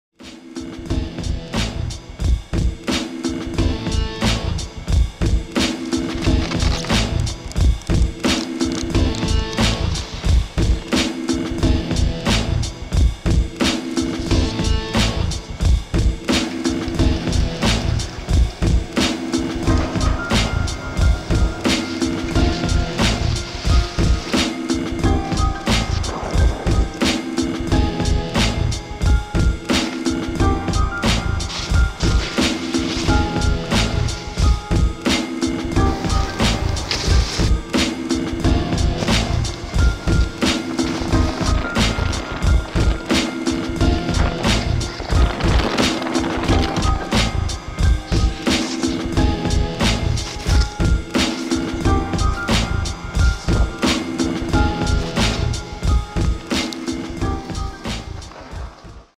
AXIS shop rats after work laps
A few laps after work with a few of our young staffs. Don't even need a park to have fun...